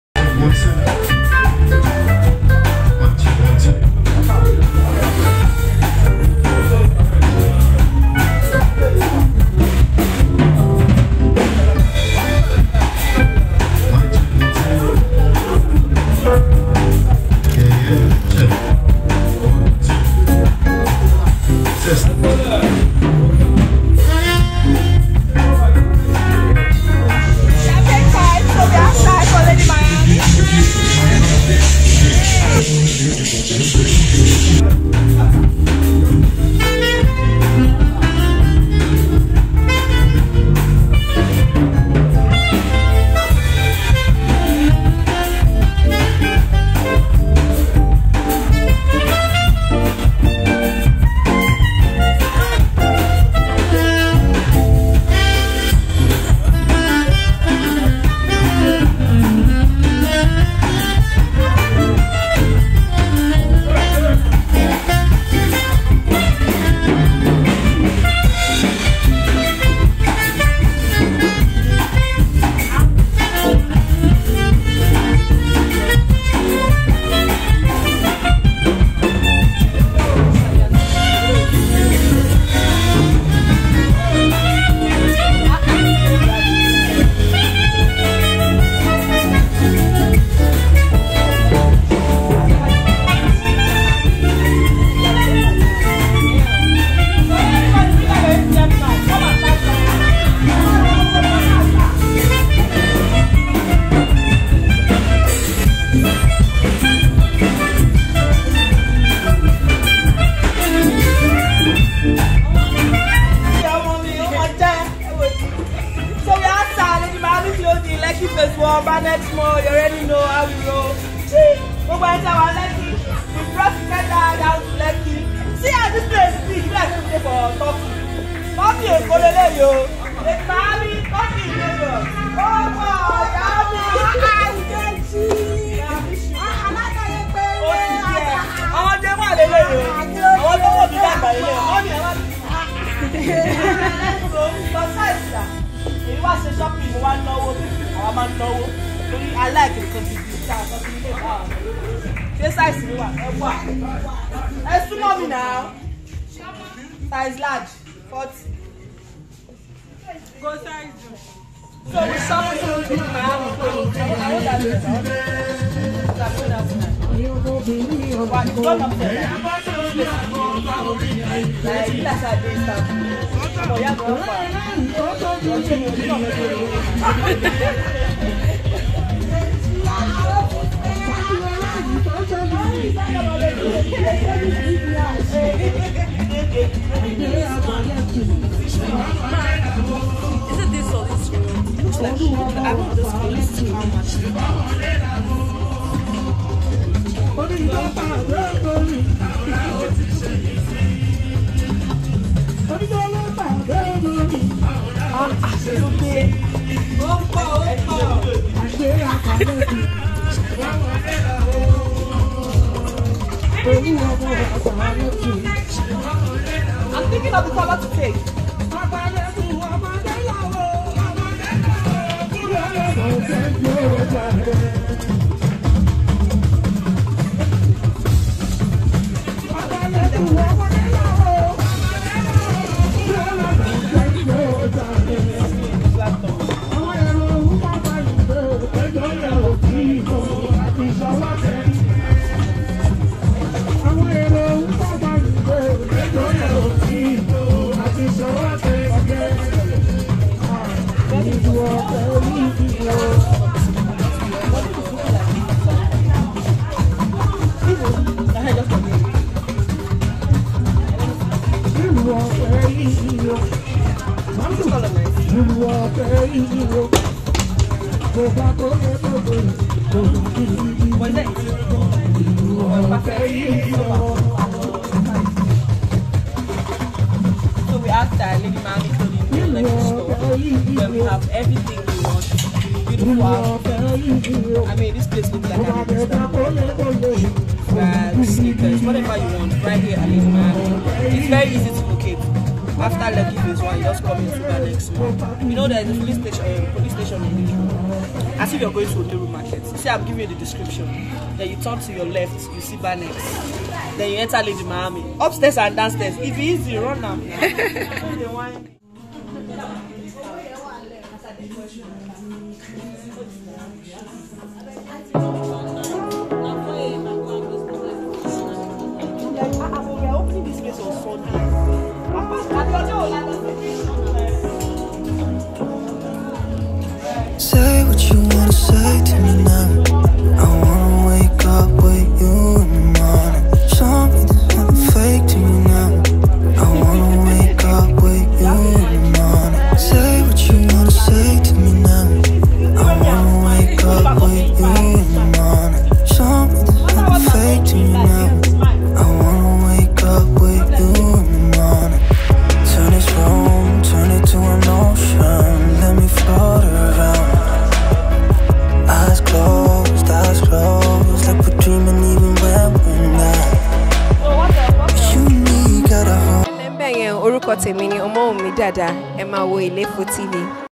GRAND ENTRANCE OF ACTRESS NKECHI BLESSING, SINDODO TAYO TO OFFICIAL OPENING OF LADY MIAMI STORE
GRAND ENTRANCE OF  ACTRESS NKECHI BLESSING, SINDODO TAYO TO OFFICIAL OPENING OF LADY MIAMI CLOTHING STORE